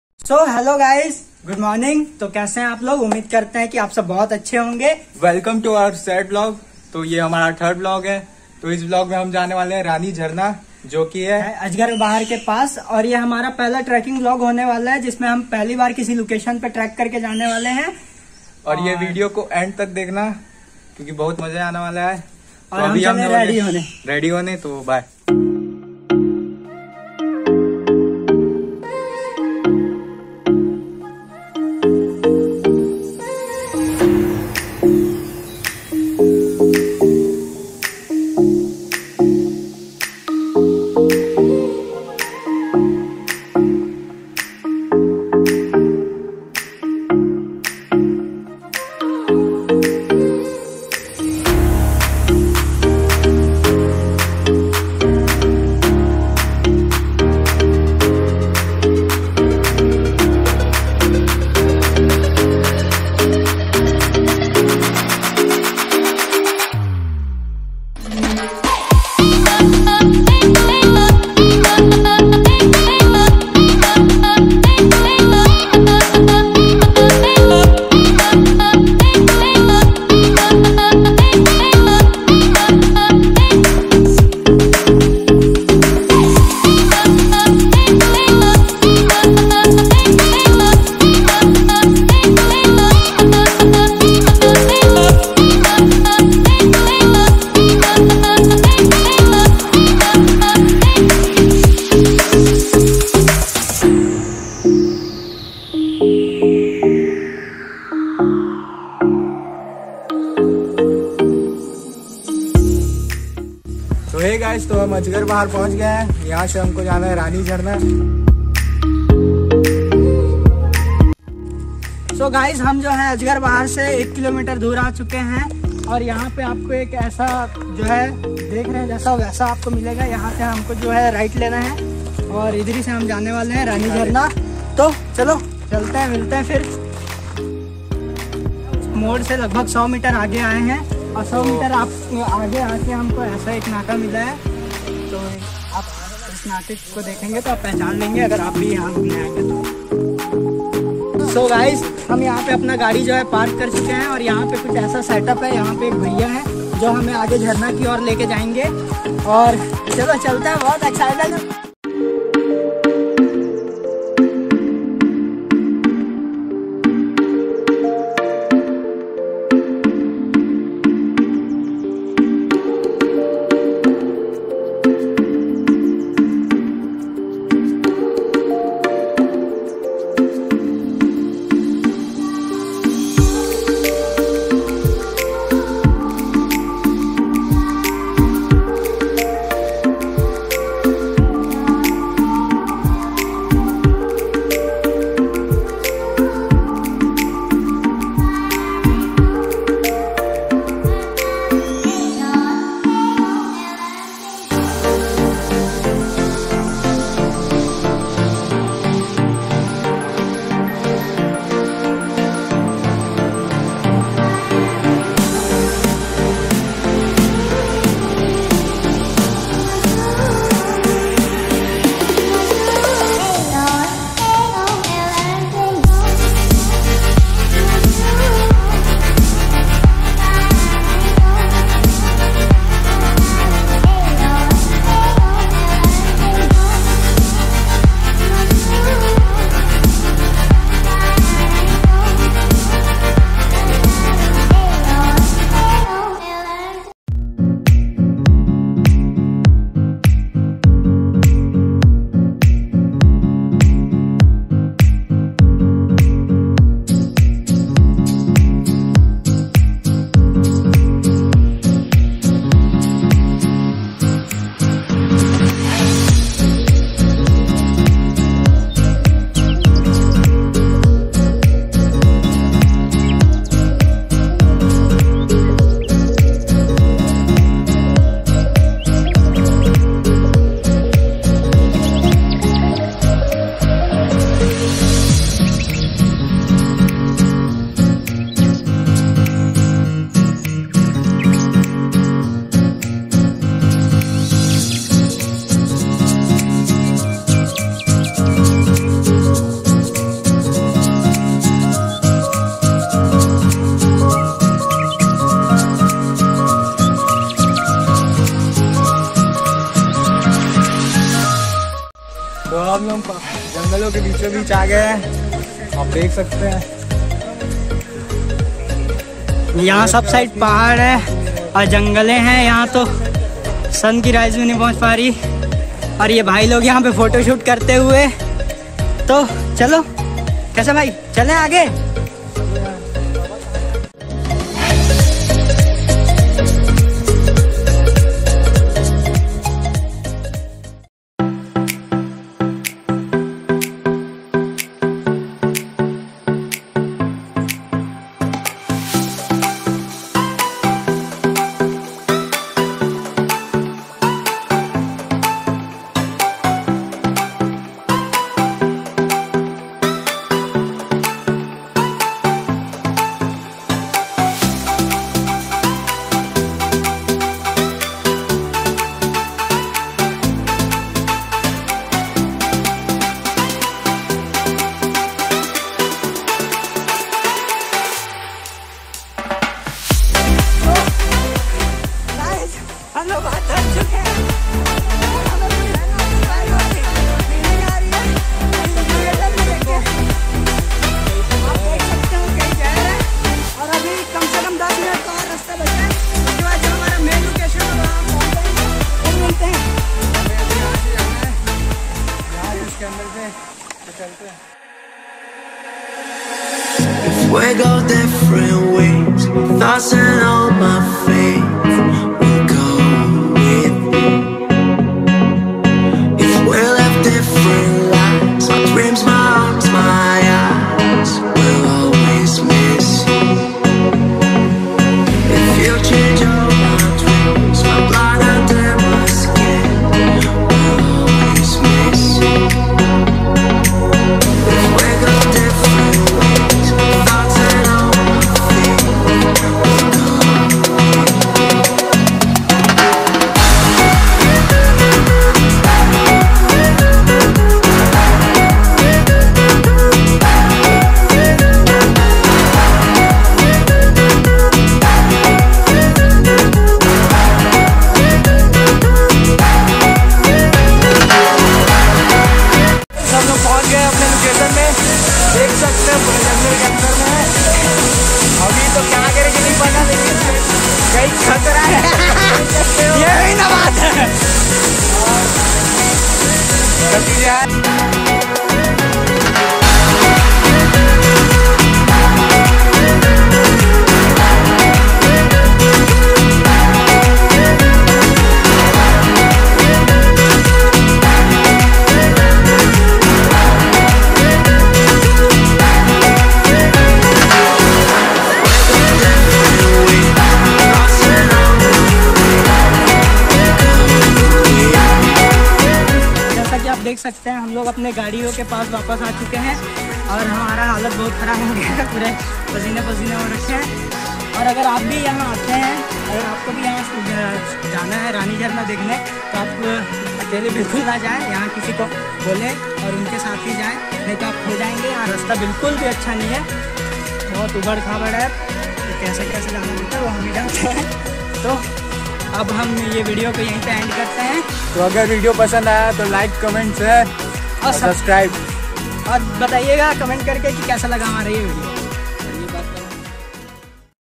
R A N I J H A R I Y A || picnic spot korba chhattisgarh detailed view TRACKING VLOG
The beauty of chhattisgarh. Rani jhariya ajgarbahar balco satrenga road. beauty of nature nature love. Tracking vlog. Nature view KEEP THE PLACE CLEAN... CLEAN INDIA GREEN INDIA.